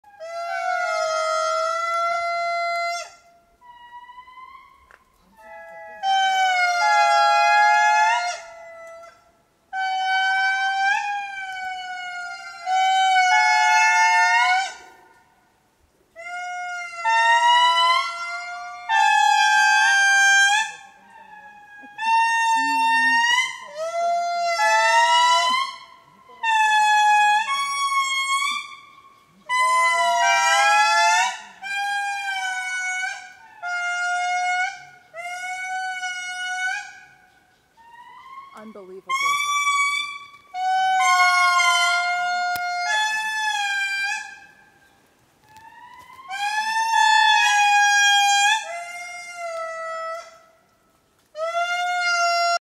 Indri calling.
A male Indri indri named Rambo calling in Mitsinjo park in Madagascar. You can hear some of his family calling offscreen.

Recorded using a Sony A7R2 and Loxia 50mm lens.